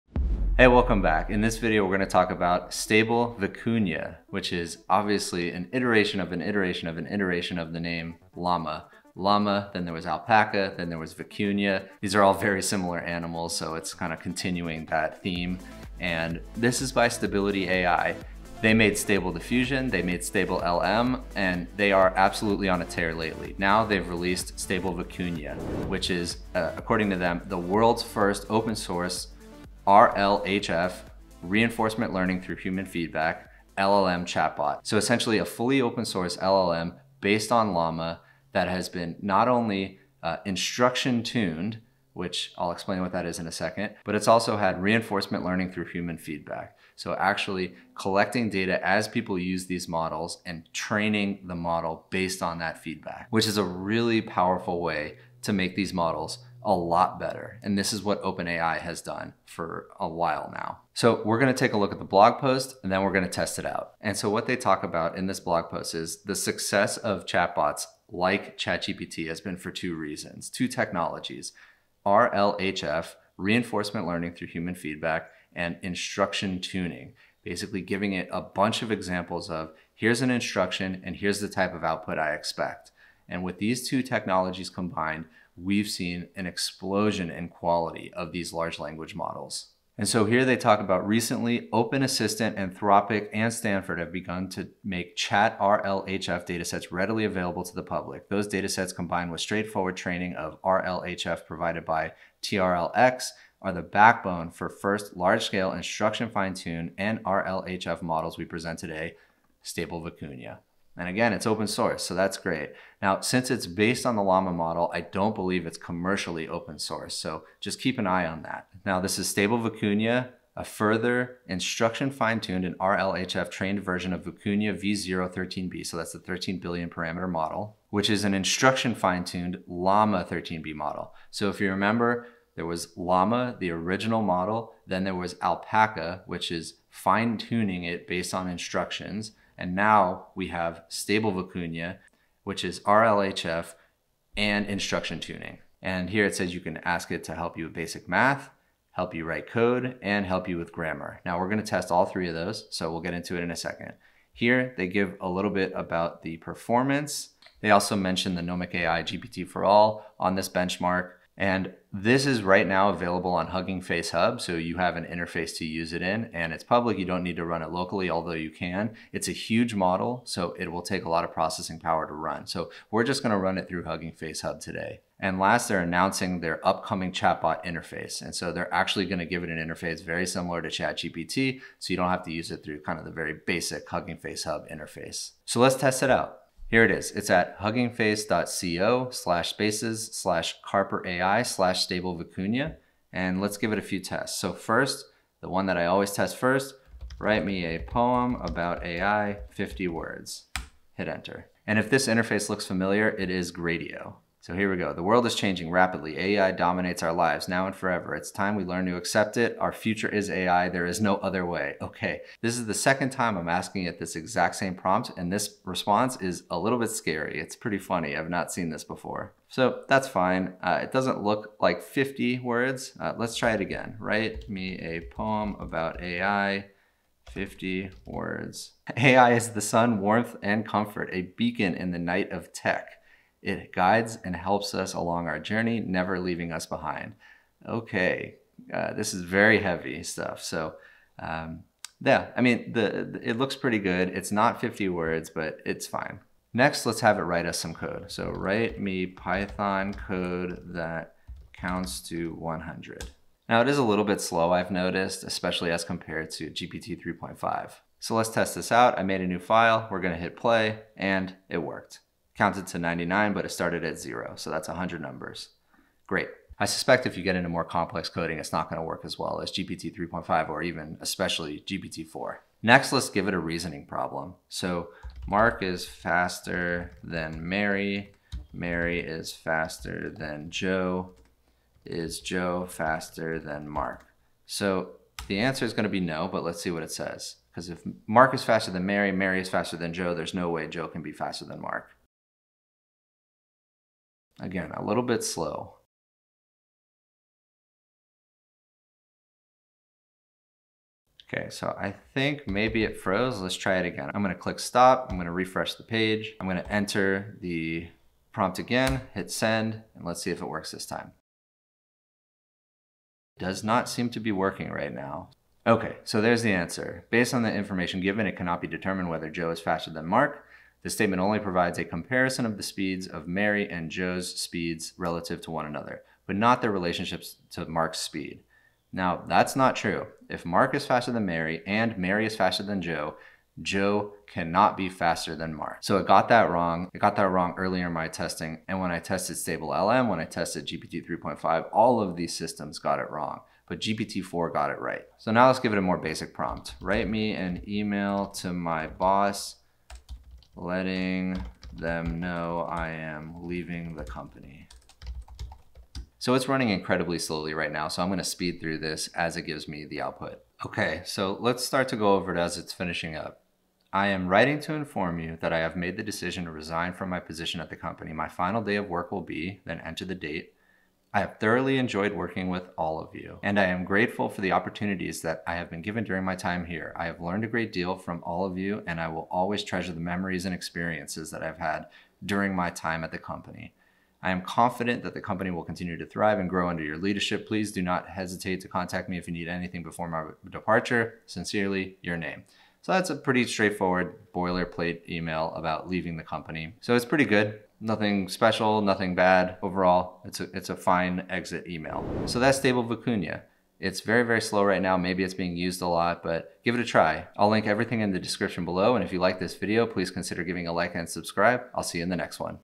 NEW Stable Vicuña - World's First RLHF LLM Chatbot
In this video, we review and test the newly released Stable Vicuña by Stabability.ai, the company behind Stable Diffusion and other AI products. This large language model is the first open-source AI to feature instruction-tuning AND RLHF (reinforcement learning through human feedback). How are the results? Let's find out!

Contents of Video:
0:00 - Intro
0:26 - Announcement Review
3:06 - Testing
6:42 - Outro